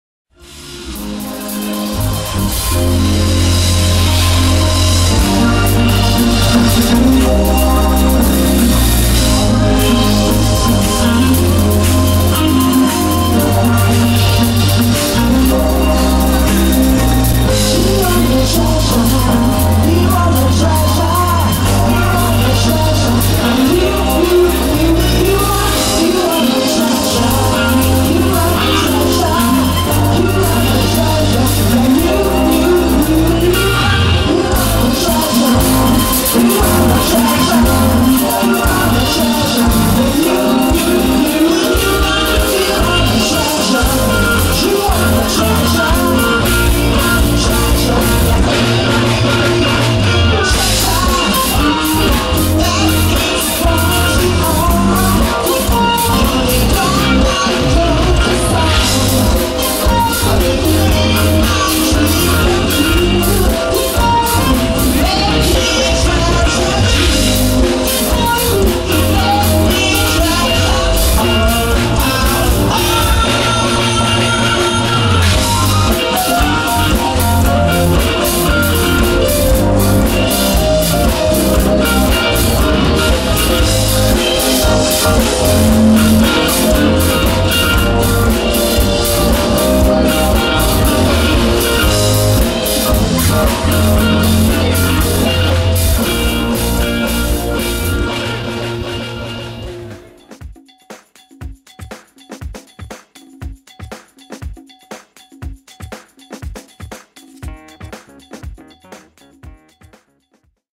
Banda Baile BH Disco Funk Festa de Santo Antonio 2017
Rio Acima Minas Gerais17-06-17 Festa de Santo Antonio Banda Dubhayle a Banda de Baile mais animada de BH Festas Shows e eventos BH MG